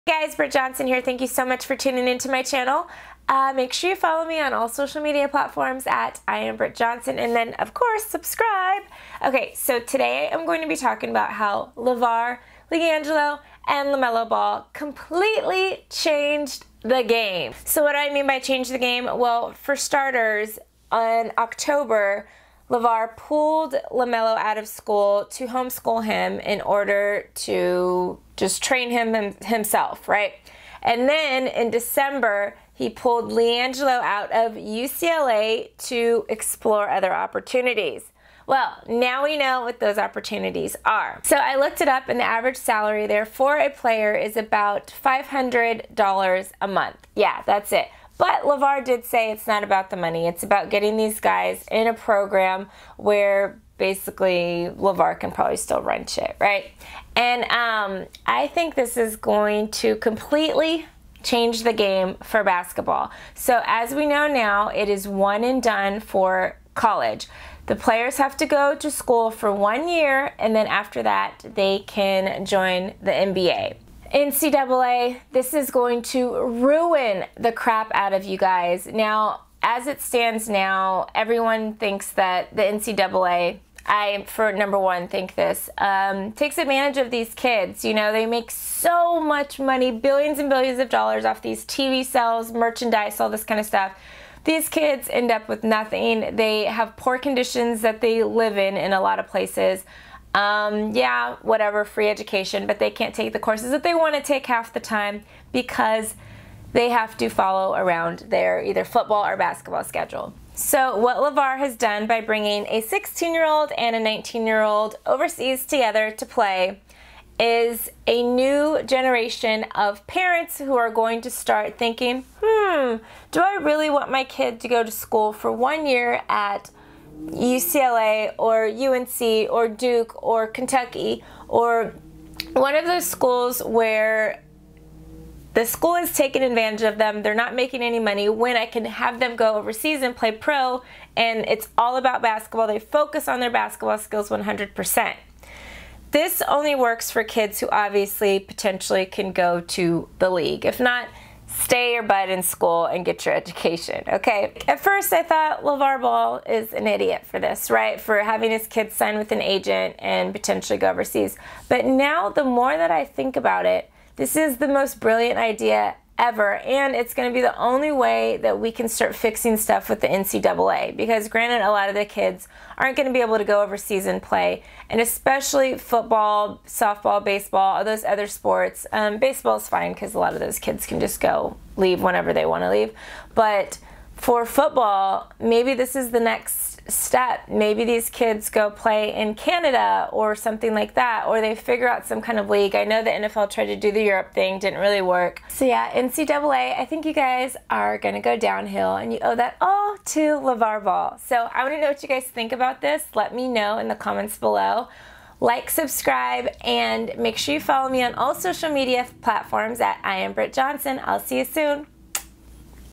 The Ball Boys just CHANGED the Game!
Britt Johnson talks about Lavar Ball taking his two youngest sons out of school to play pro ball overseas. Was this a good move and how will it affect the future of the NCAA.